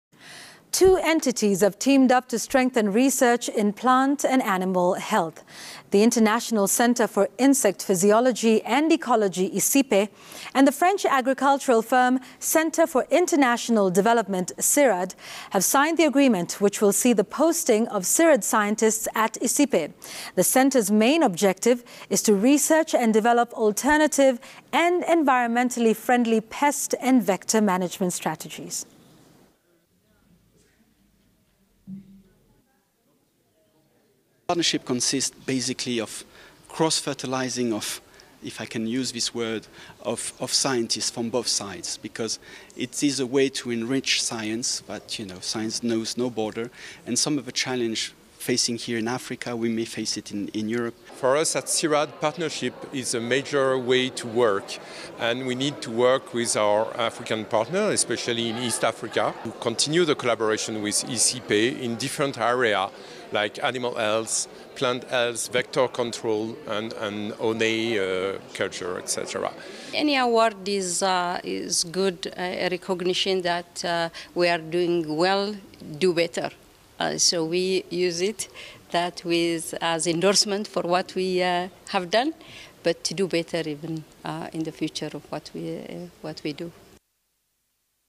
ICIPE and CIRAD in agreement to boost research work
he International Centre for Insect Physiology and Ecology ICIPE and the French Agricultural Research Centre for International Development CIRAD have signed the agreement which will see the posting of CIRAD scientists at ICIPE. The centre's main objective is to research and develop alternative and environmentally friendly pest and vector management strategies.